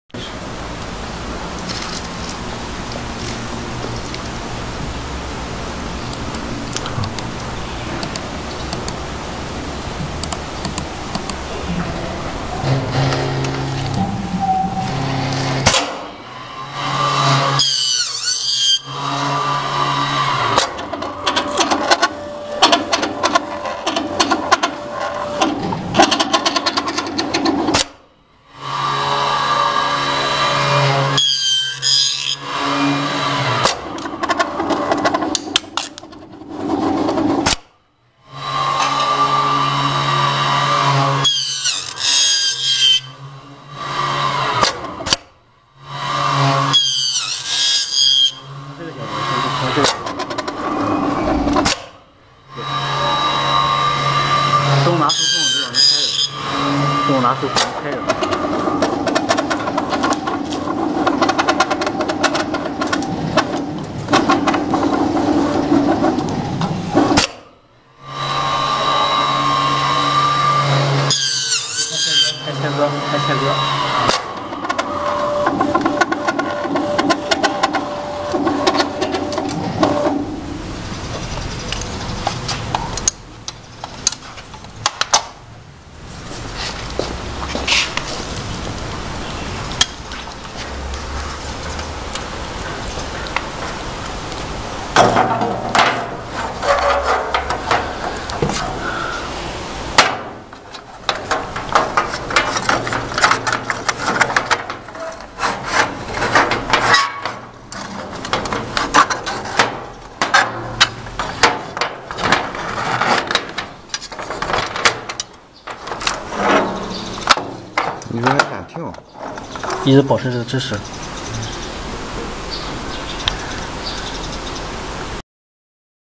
Aluminum Channel Letter Bending machine
This machine is special aluminum profile bending machine, its precision can be 0.1mm, you can make it endless channel letters.